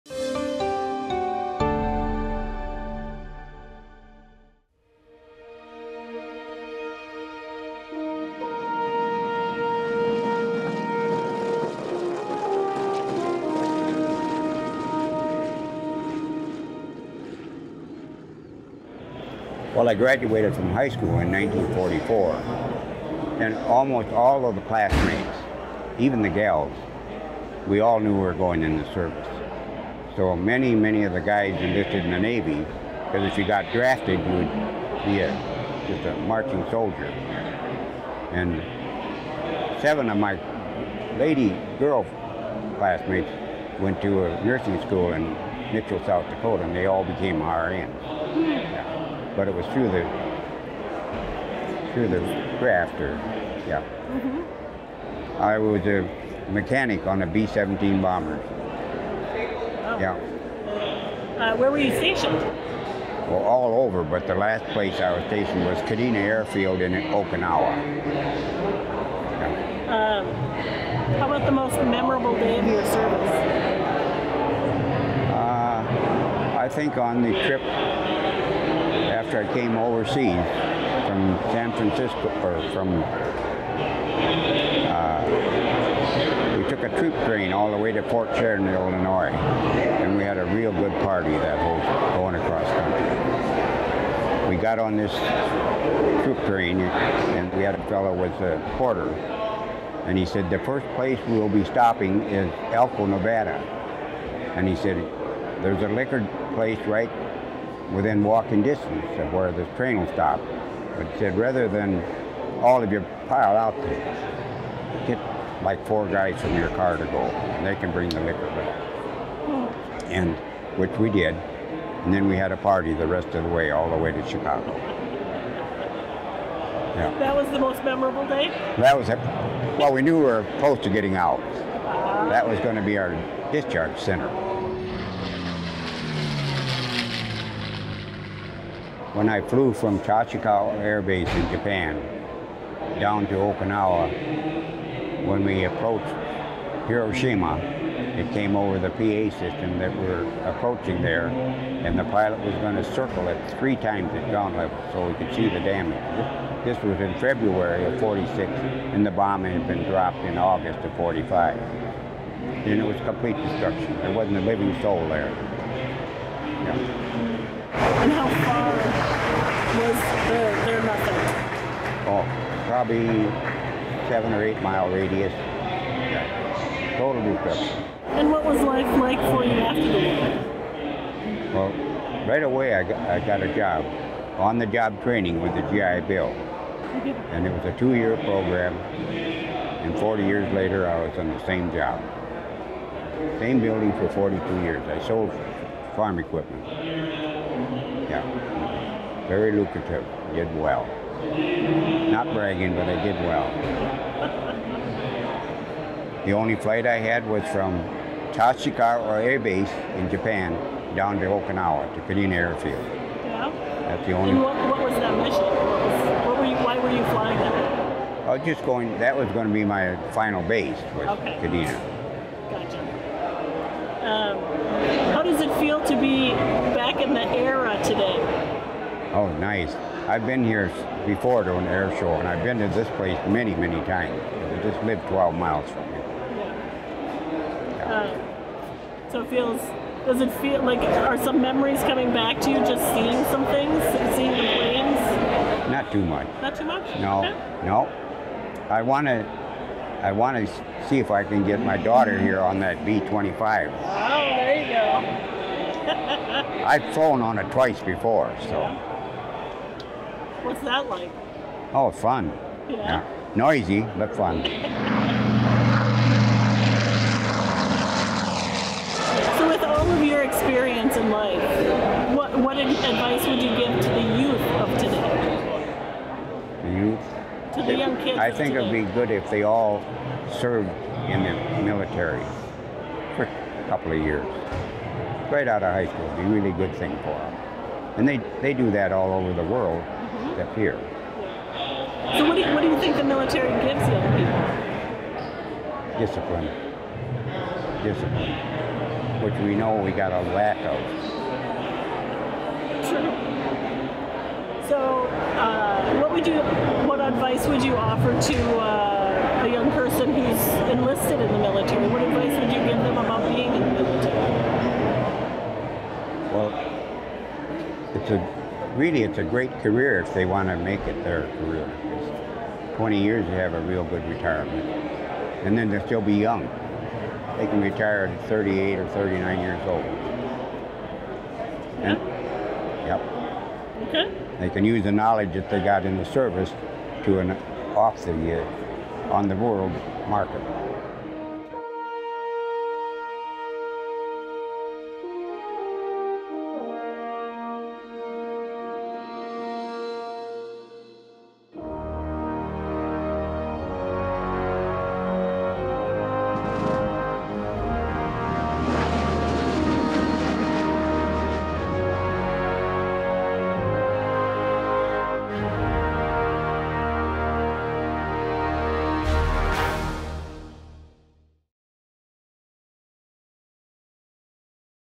William Homan, WWII Oral History
William Homan is a WWII US Air Corps Veteran. He was a mechanic on a b-17 bomber and witnessed the aftermath of the atomic bomb on Hiroshima  months after it was dropped.